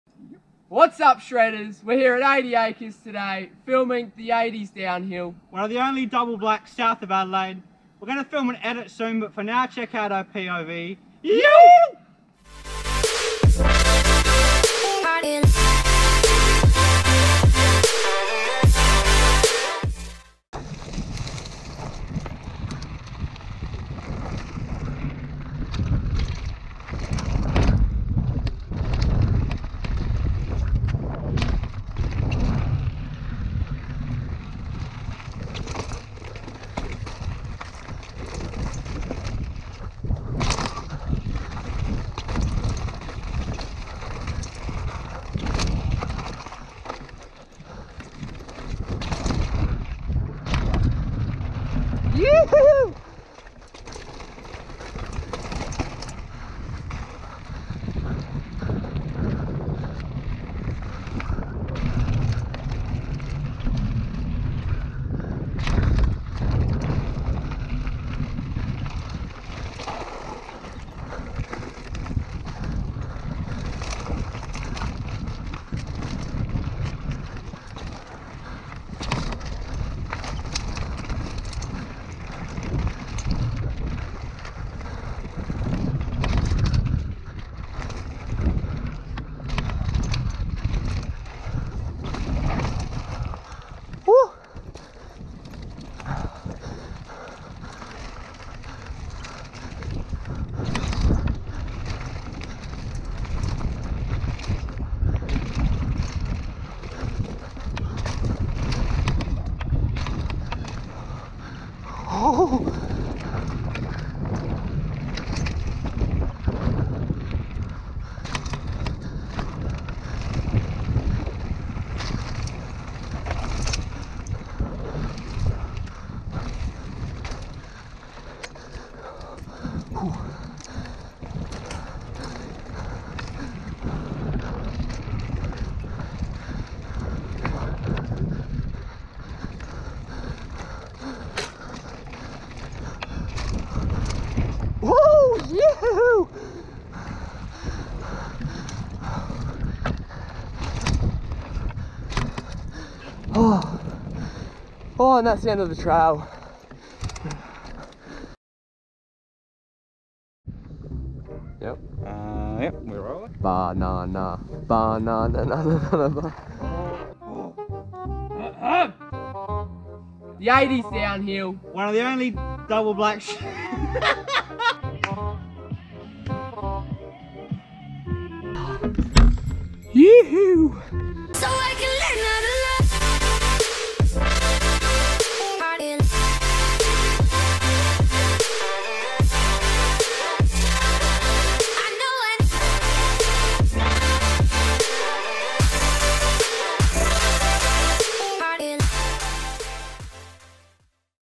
80's Downhill | 80 Acres
POV of one of Southern Adelaide's double blacks.

Next weeks video will be another POV of O'Halloran Hill